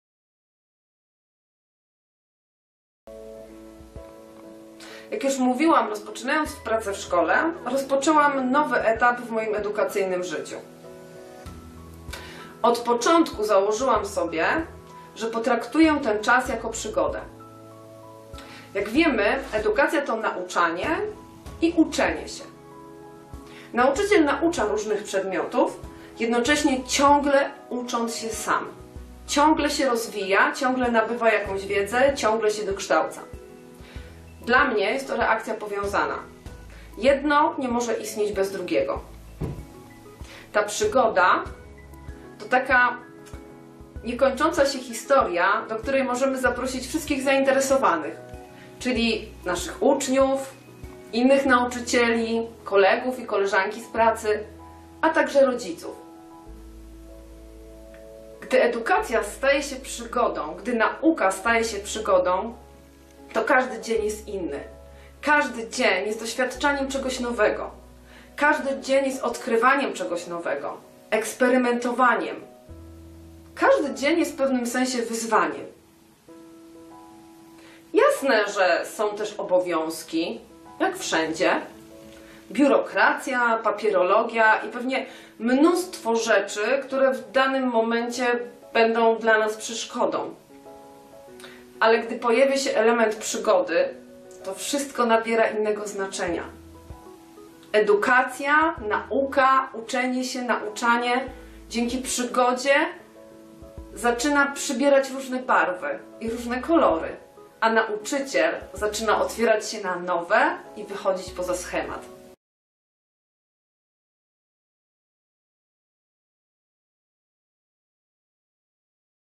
Satysfakcja w pracy nauczyciela cz.2: przygoda
Marta Florkiewicz-Borkowska opowiada o przygodzie w pracy nauczycieli i nauczycielek w ramach Spółdzielni Otwartej Edukacji.